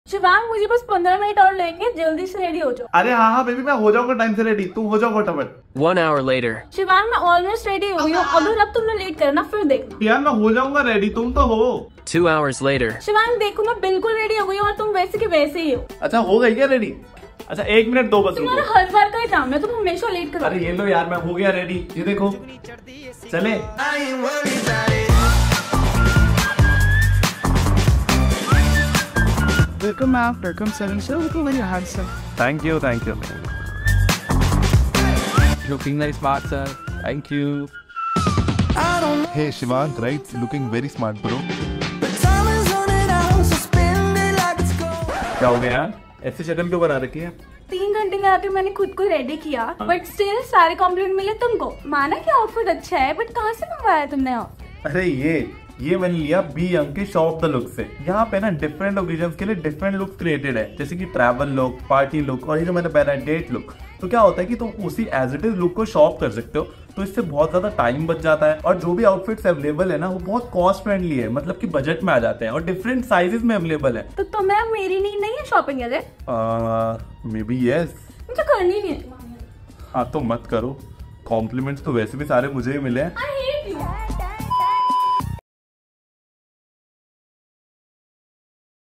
Shop The Look and Turn Heads Everywhere - Beyoung
Discover the secret to effortless style and endless compliments with Beyoung Shop The Look. Elevate your fashion game with looks for every event that is sure to turn heads wherever you go. Unlock your fashion potential and step into a world of compliments with Beyoung's stylish collection.